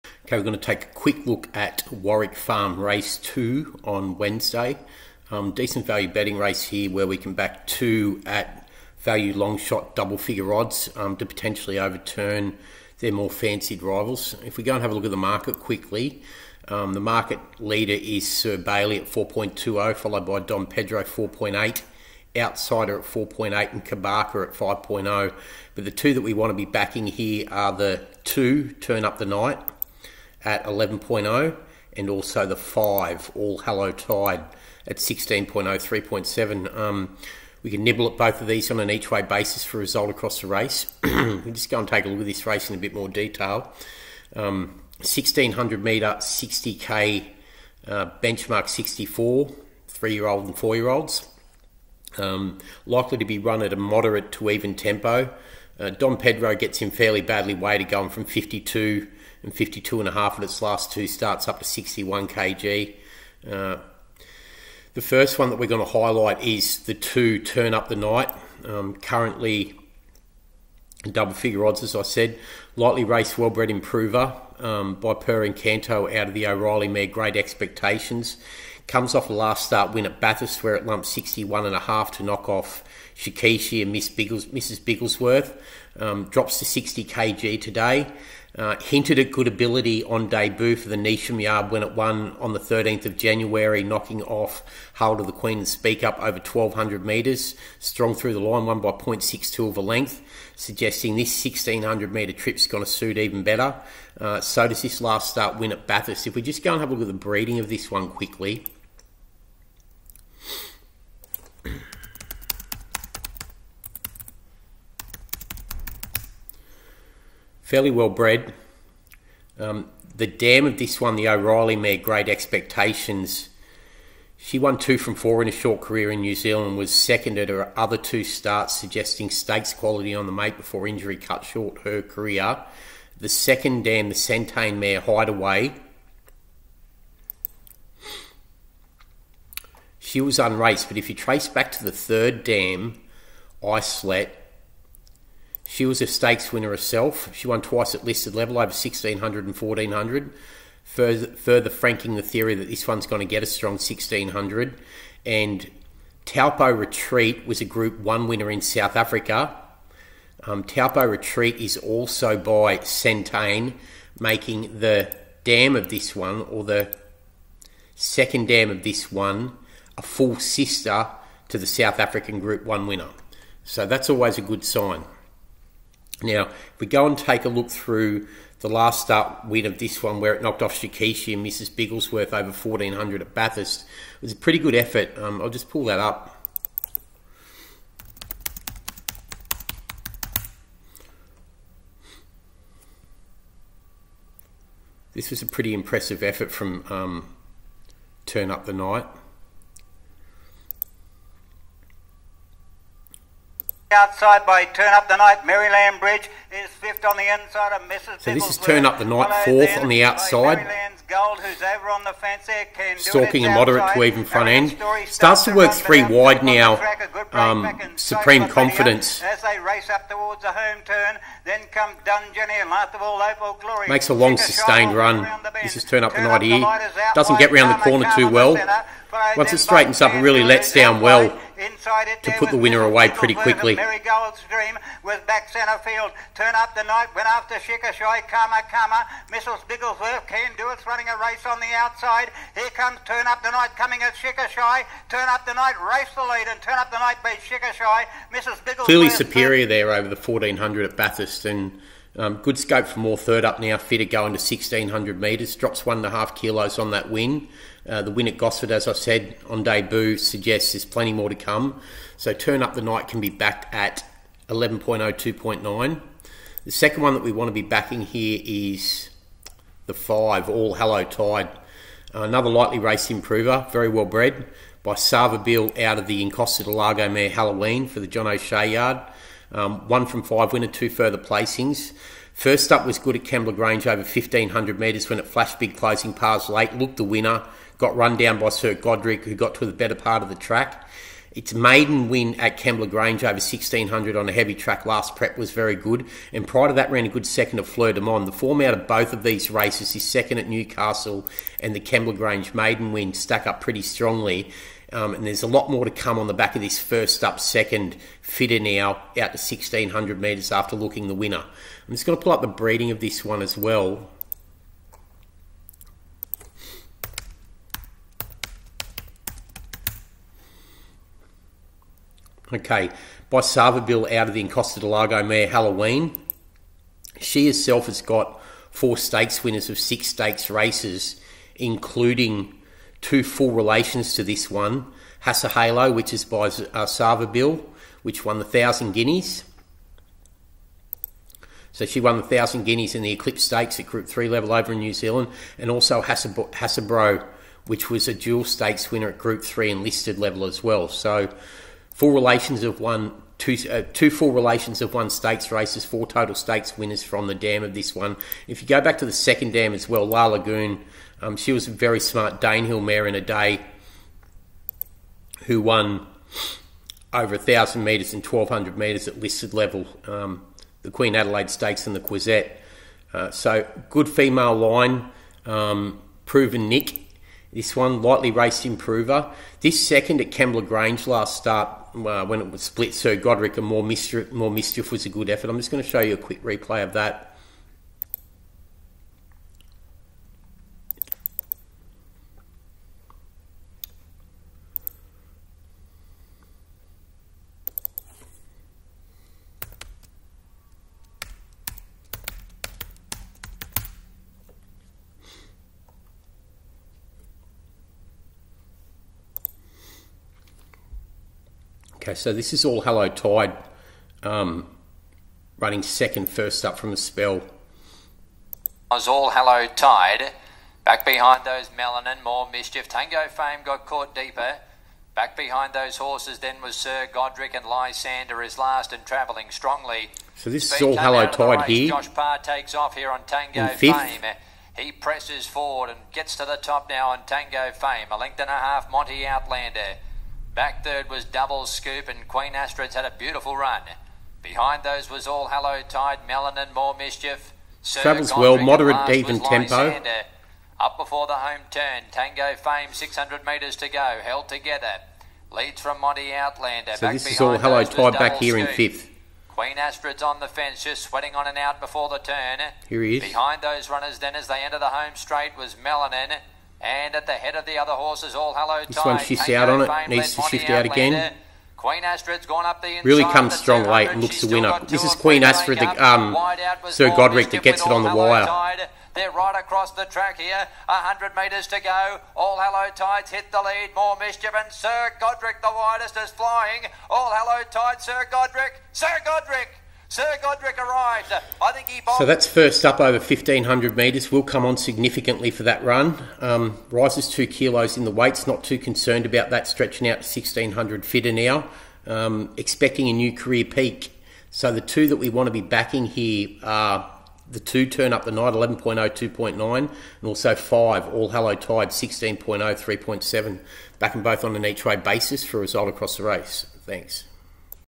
Warwick Farm Race 2 Preview - Wednesday 9 August by The Winning Factor
Good value betting affair where we feel the 4 market leaders can be opposed, we will be backing 2 runners at Value Longshot odds that are both well bred lightly raced improvers which look set for new career peaks here.

For more high quality betting intelligence on a daily basis go